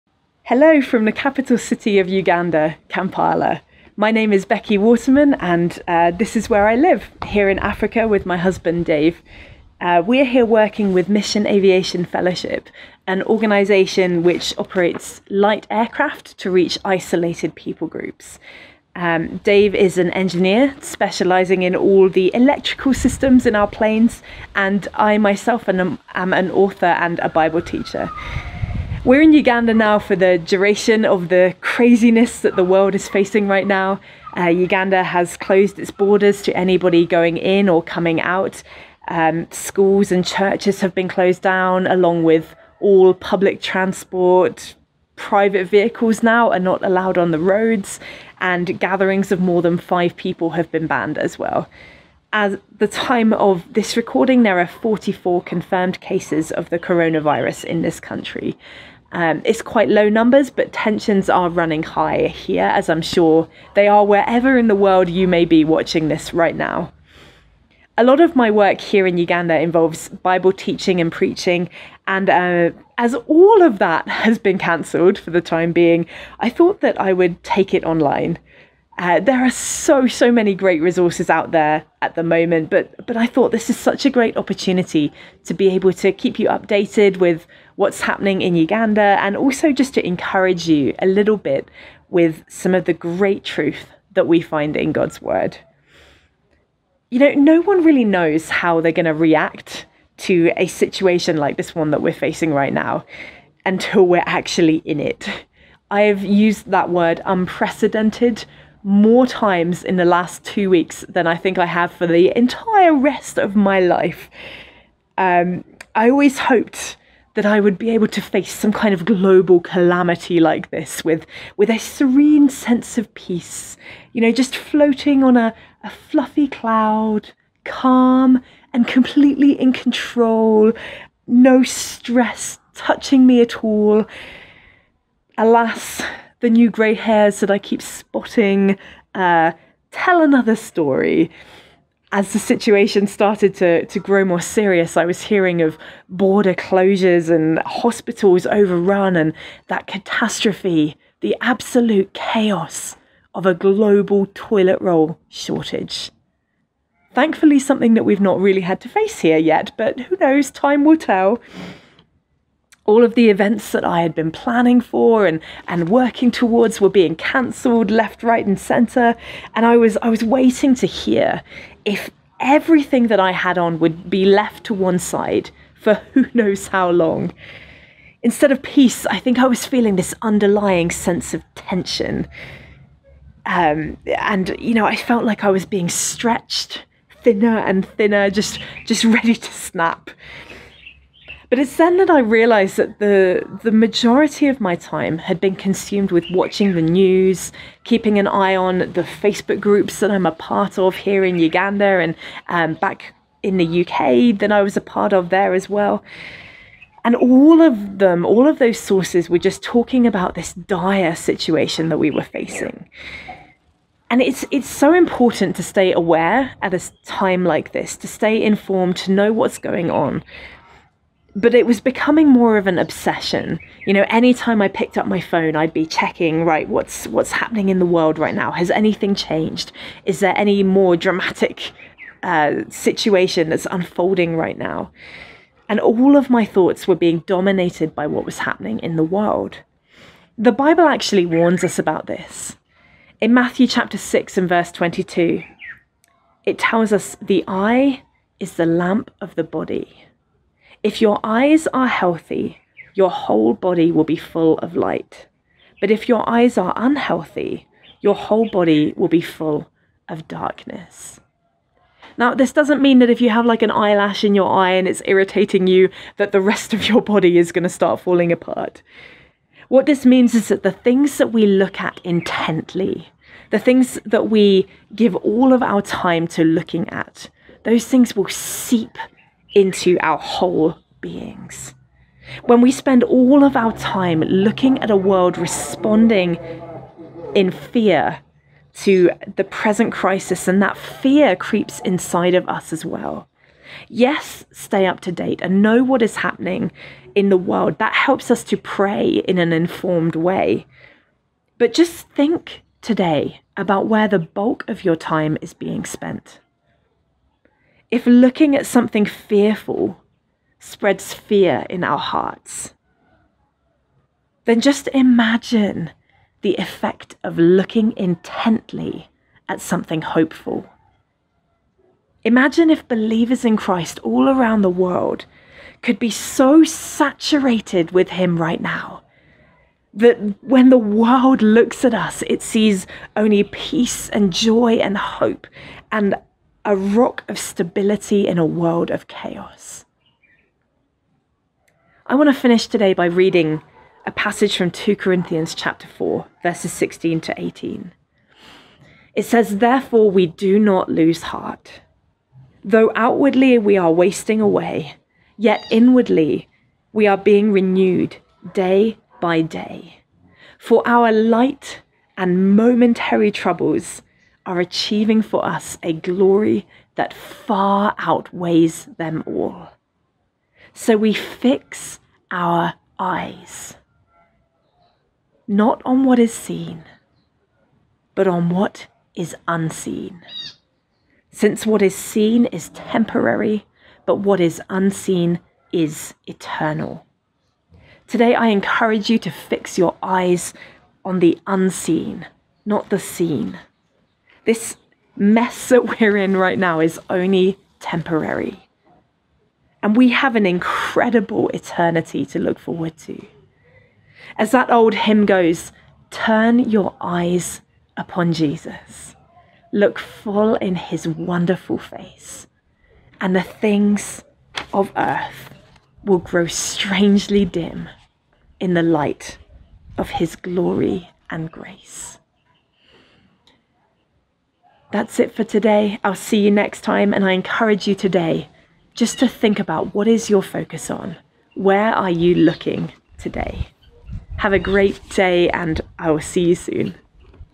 What's Your Focus? | Becky's Bible Thoughts
What's your focus? What do you spend most of your time looking at right now? In this first teaching video, Becky Waterman, MAF missionary, explores how important it is to consider what you are giving the highest priority to in these troubled times.